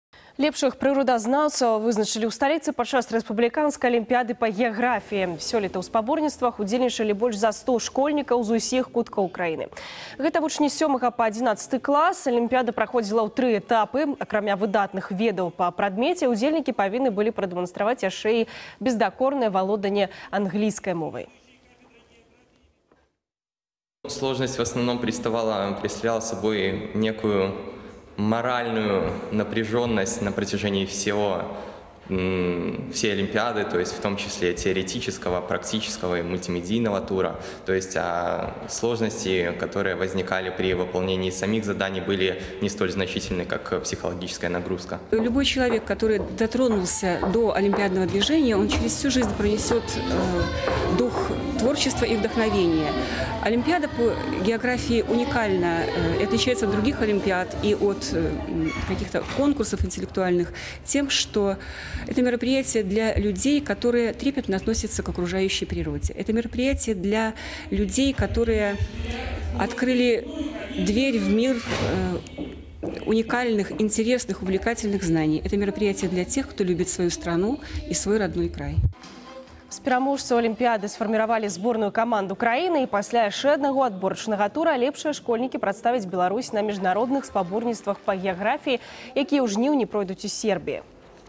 Лепшых прыродазнаўцаў вызначылі падчас рэспубліканскай алімпіяды па геаграфіі
У спаборніцтвах удзельнічалі больш за 100 школьнікаў з усіх куткоў краіны. Гэта вучні з 7 па 11 клас. Алімпіяда праходзіла ў тры этапы. Акрамя выдатных ведаў па прадмеце, удзельнікі павінны былі прадэманстраваць яшчэ і бездакорнае валоданне англійскай мовай.

З пераможцаў алімпіяды сфарміравалі зборную каманду краіны. І пасля яшчэ аднаго адборачнага тура лепшыя школьнікі прадставяць Беларусь на міжнародных спаборніцтвах па геаграфіі, якія у жніўні пройдуць у Сербіі.